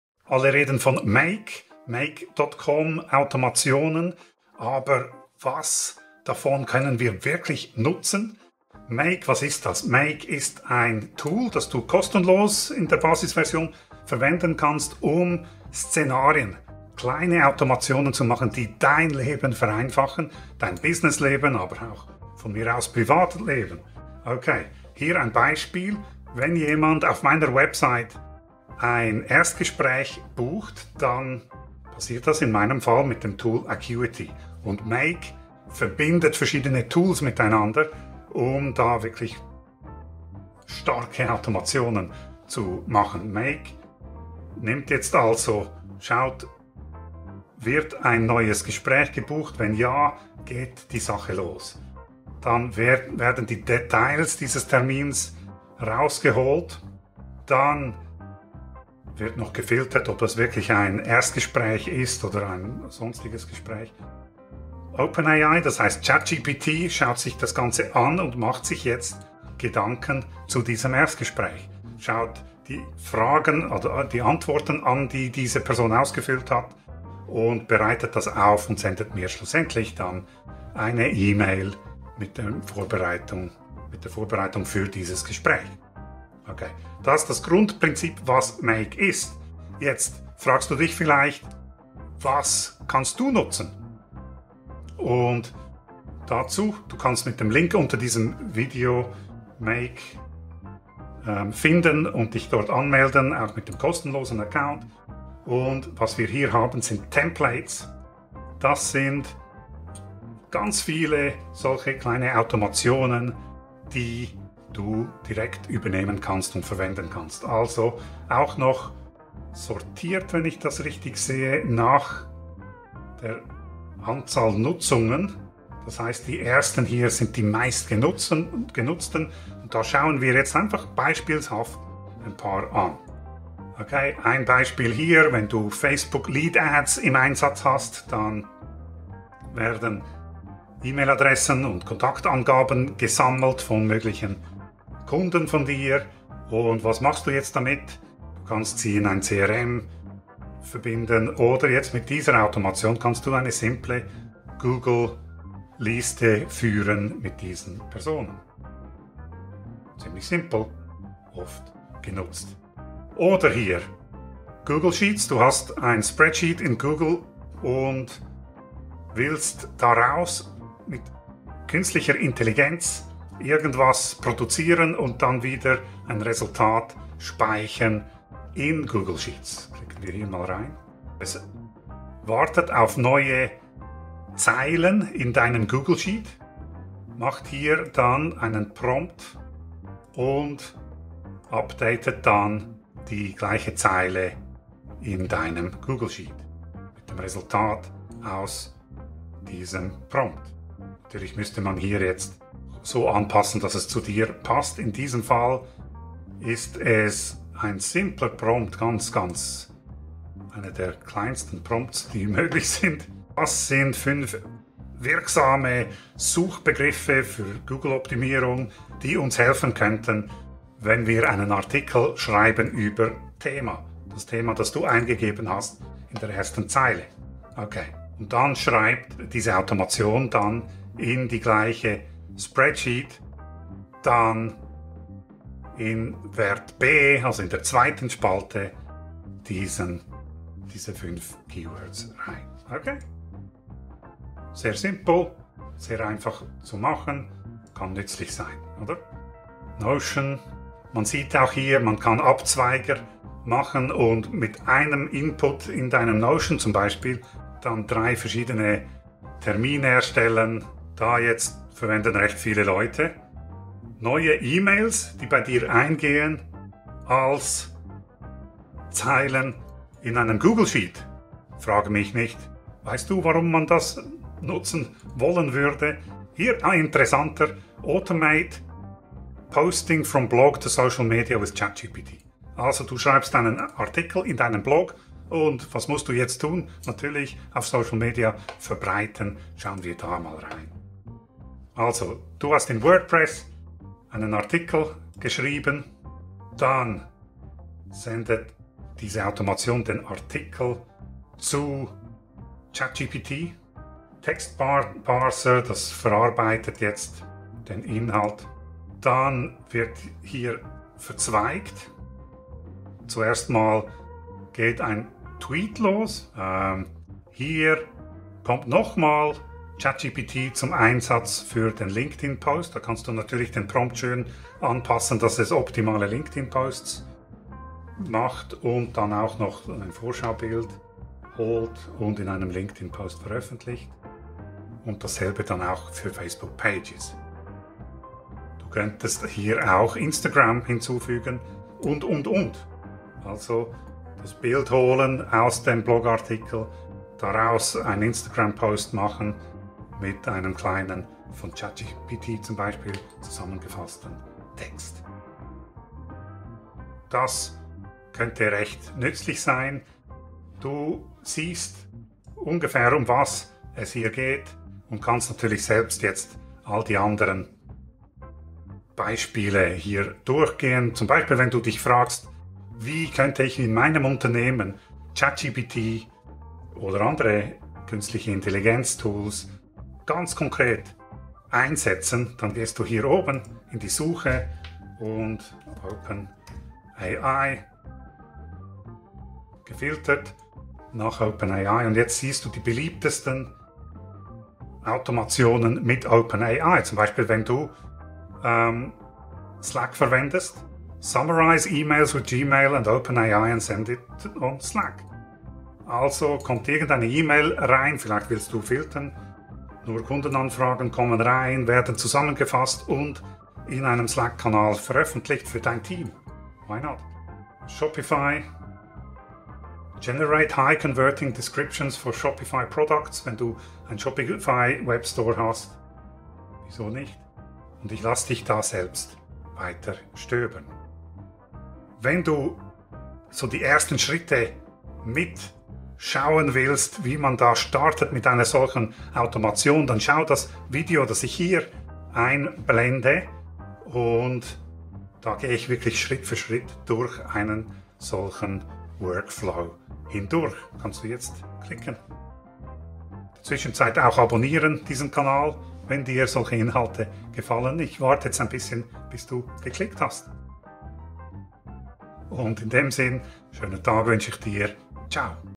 4 NÜTZLICHE Automationen mit K.I. für Selbständige
Ein Überblick über das Automationstool Make.com und 4 Beispiele, die du direkt in deinem Business verwenden kannst.

Ich helfe dir, online effizient neue Kunden zu gewinnen.
Damit du mehr bewirkst. Und das relaxter.